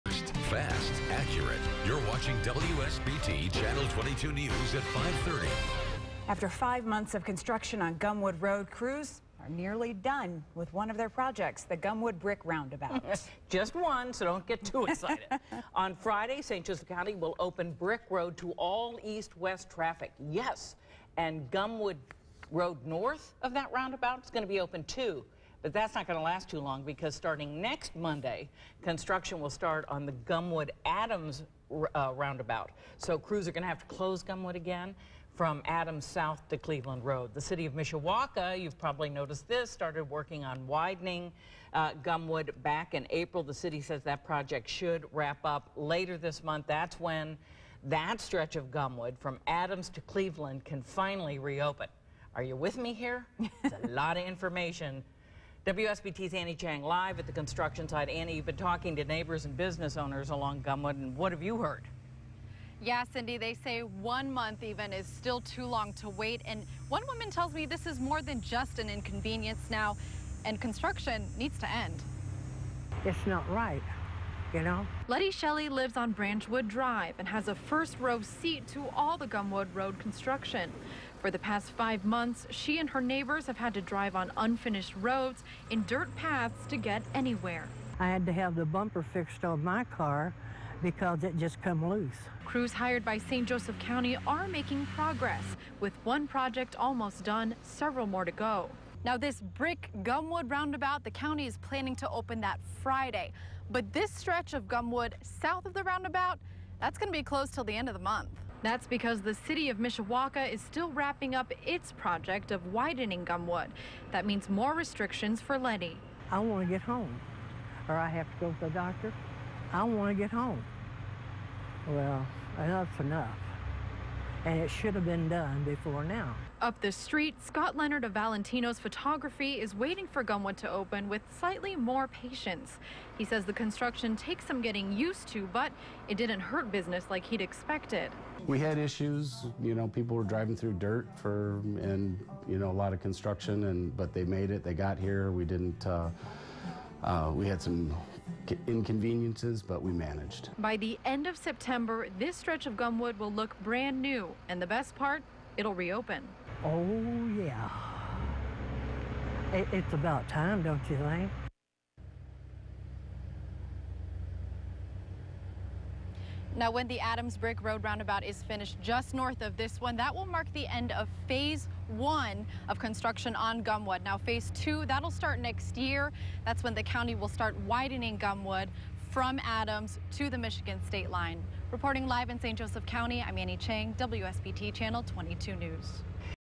Crews nearly done with Gumwood-Brick roundabout
After five months of construction on Gumwood Road crews are nearly done with one of their projects: the Gumwood-Brick roundabout. _________________